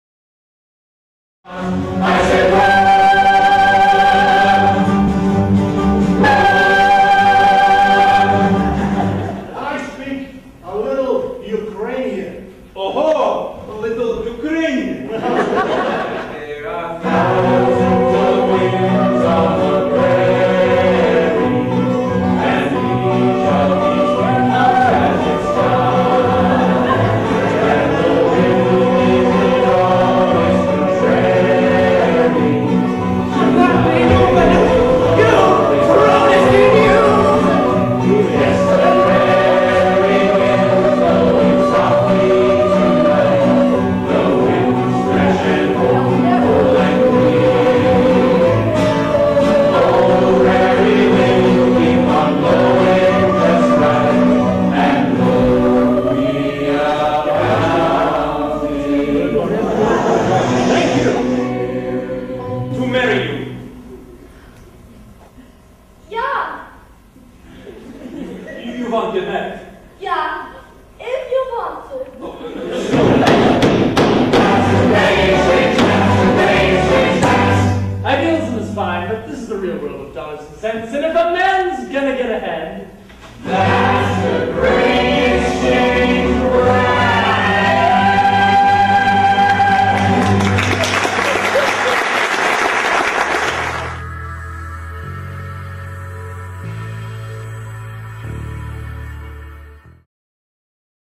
Paper Wheat
Excerpts from our 2009 production of 25th Street House Theatre's Paper Wheat directed by Clint Hames, former mayor of Chilliwack. A play about the early settlers of the Canadian Prairies and the life of the Co-operative Commonwealth Federation, it features students JD Dueckman, Caroline Davies, Tim Howe, Cassandra Tracy and Joel Colbourne as well as the cast from our first production of Paper Wheat in the 1980s: Rick Mawson, Lynne Wells, Ian Fenwick and Sabine Quilman.